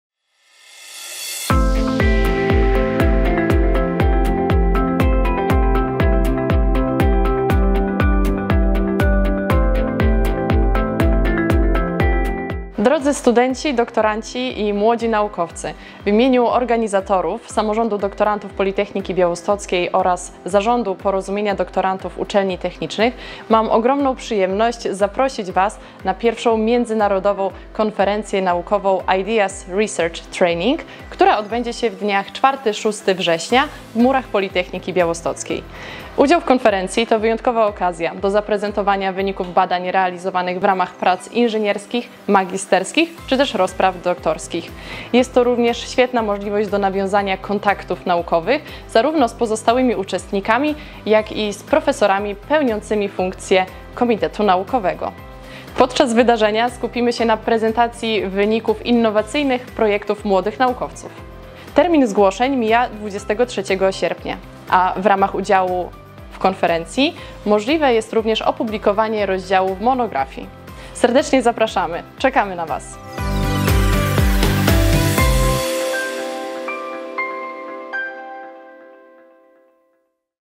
I Międzynarodowa Konferencja Młodych Naukowców w Politechnice Białostockiej!
I Międzynarodowa Konferencja Młodych Naukowców: Pomysły, Badania, Szkolenia, czyli 1st International Young Researchers Conference: Ideas, Research, Training – odbędzie się w Politechnice Białostockiej w dniach 4-6 września 2024 r.

– Udział w konferencji to wyjątkowa okazja do zaprezentowania wyników badań realizowanych w ramach prac inżynierskich, magisterskich, czy też rozpraw doktorskich. To również szansa na nawiązanie kontaktów naukowych zarówno z pozostałymi uczestnikami, jak i profesorami z naszego komitetu naukowego. Uczestnictwo w wydarzeniu daje także możliwość opublikowania rozdziału w monografii. Serdecznie zapraszamy – zachęca do udziału w konferencji mgr inż. Adriana Dowbysz z Samorządu Doktorantów Politechniki Białostockiej, sekretarz Zarządu Porozumienia Doktorantów Uczelni Technicznych i jednocześnie doktorantka na Wydziale Budownictwa i Nauk o Środowisku.

Organizatorzy: Samorząd Doktorantów Politechniki Białostockiej, Porozumienie Doktorantów Uczelni Technicznych
Partnerzy: Fundacja na Rzecz Rozwoju Politechniki Białostockiej, Uczelniana Rada Samorządu Doktorantów Politechniki Śląskiej
Patronaty honorowe: JM Rektor Politechniki Białostockiej, Prezydenta Miasta Białegostoku, Marszałka Województwa Podlaskiego